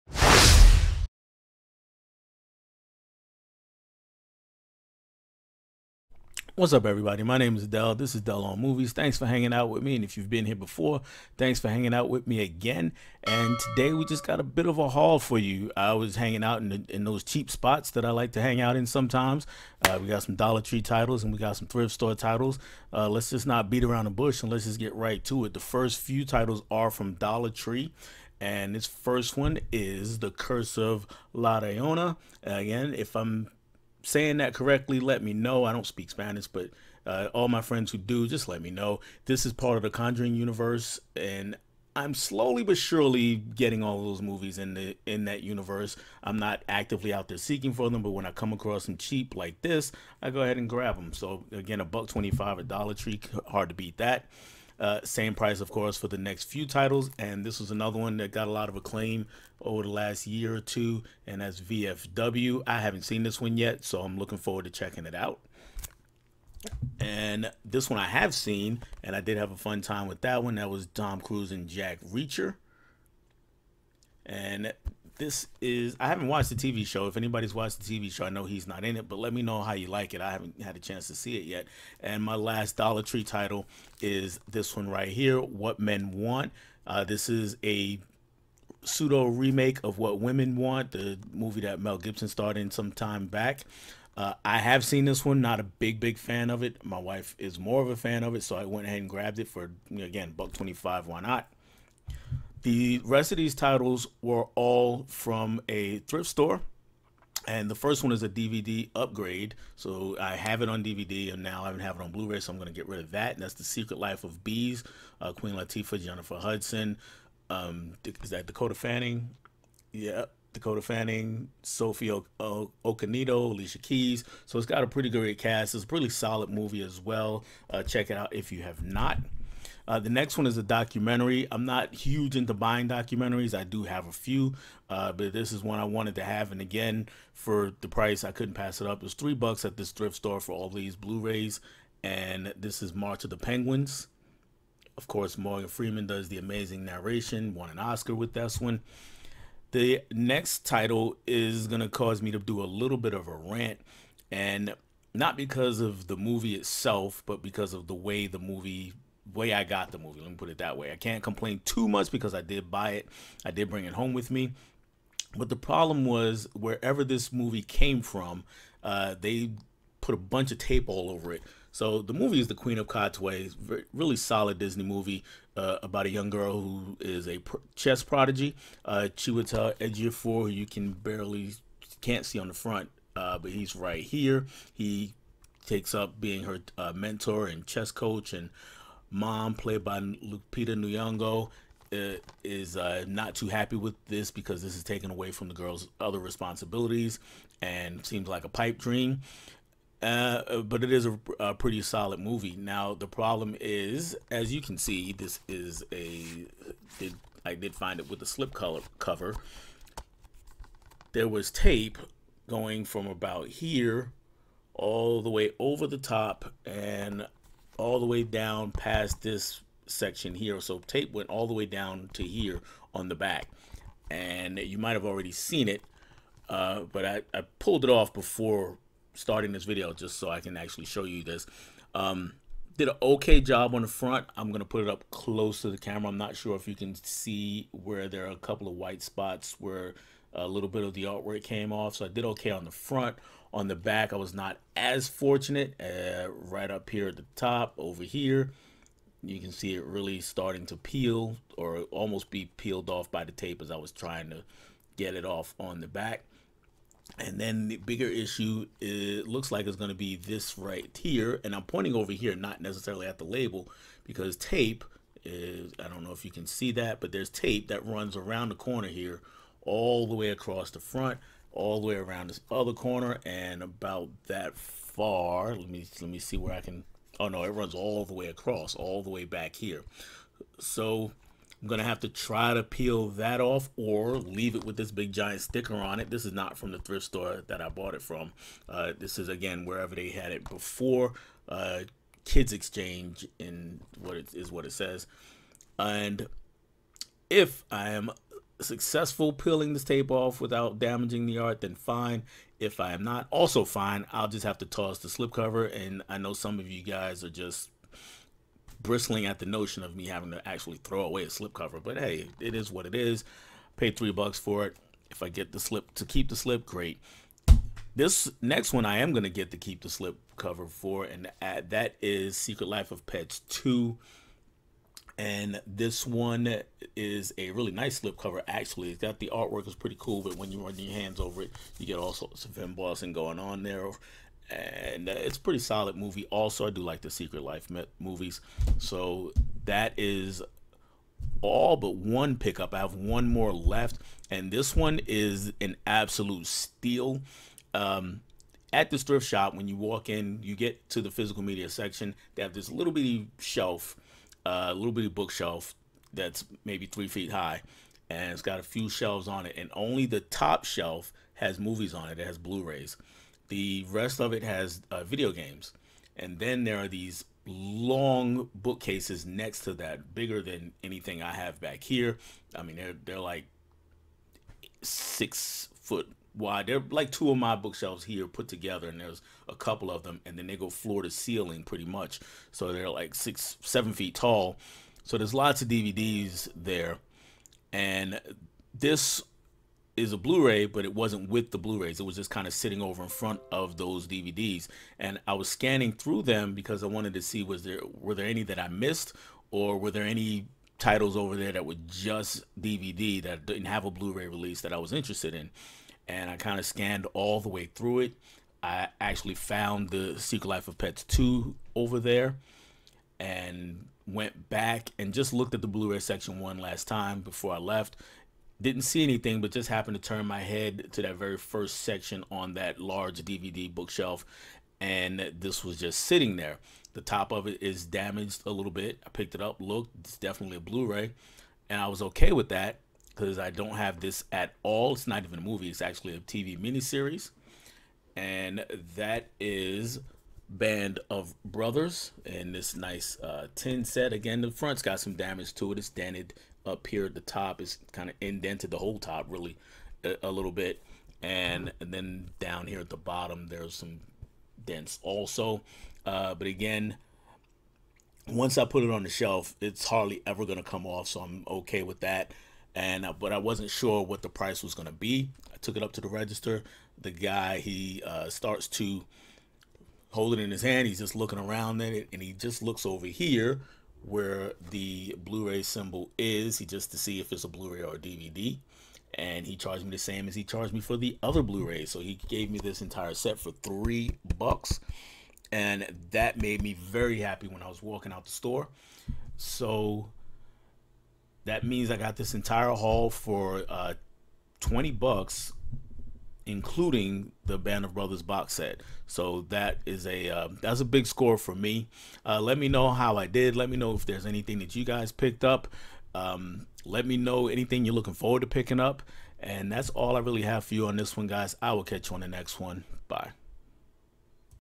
Dollar Tree/Goodwill Blu Ray Haul!!! (Early June '23)
Once again, I only spent a little bit of money to get a lot of movies. Check out what I picked up from Dollar Tree and my local goodwill store. How'd I do? Did you grab anything this week? Let me know below!